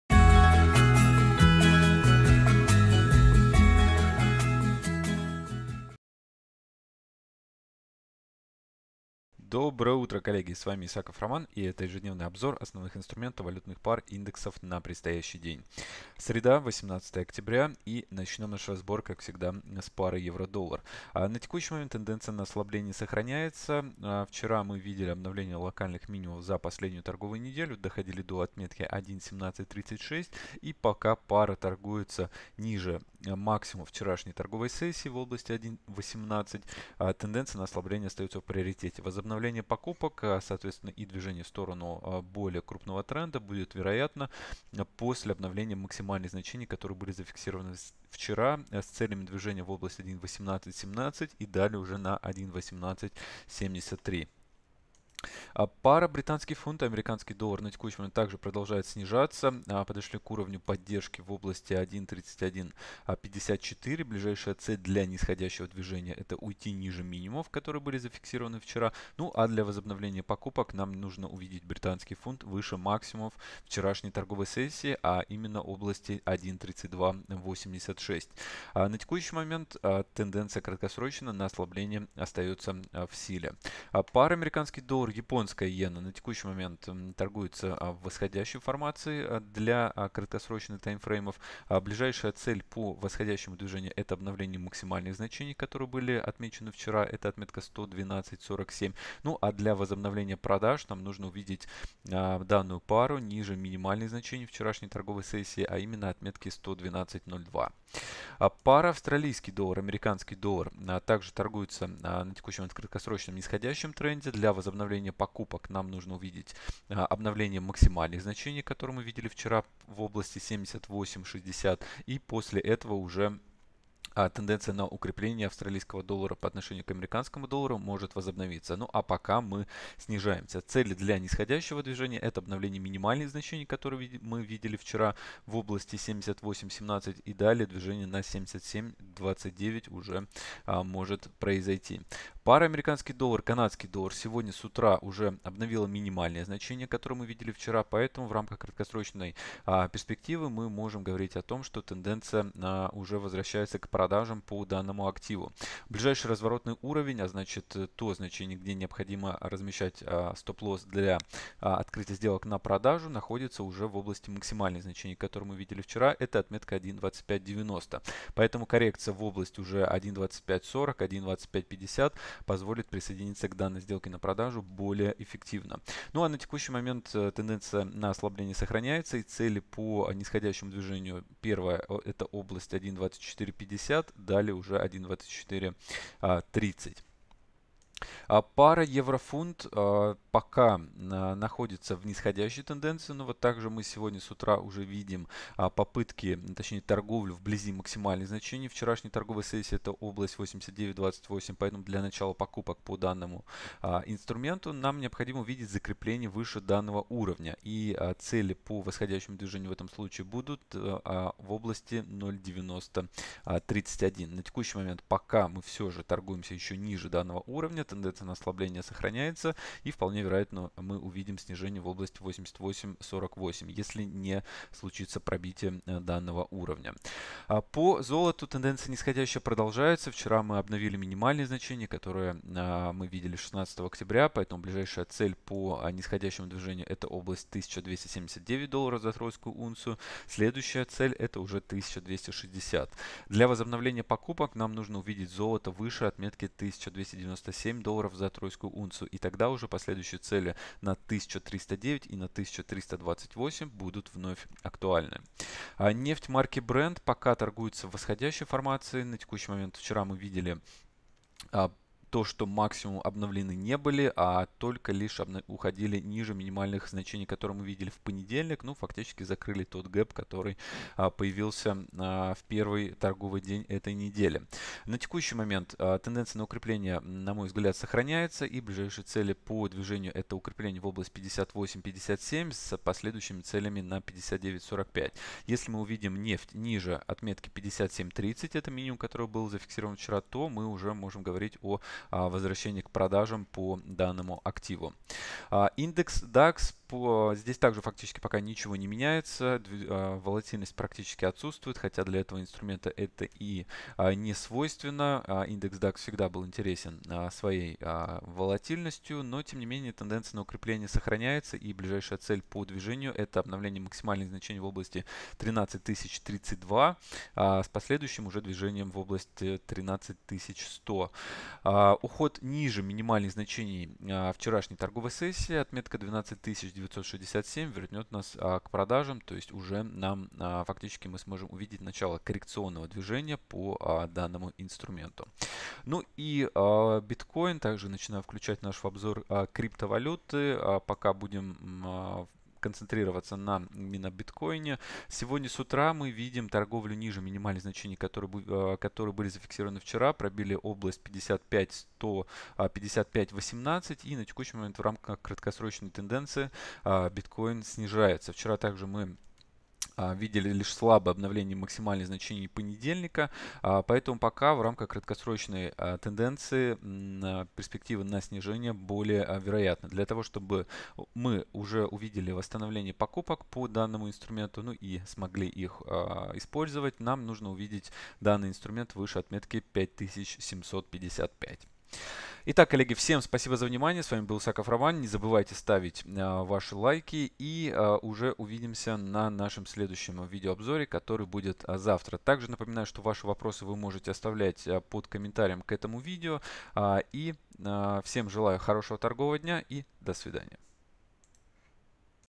Прогноз рынка форекс на основе системы Price Action с Романом Исаковым 18 октября 2017
Ежедневный обзор текущей ситуации на финансовых рынках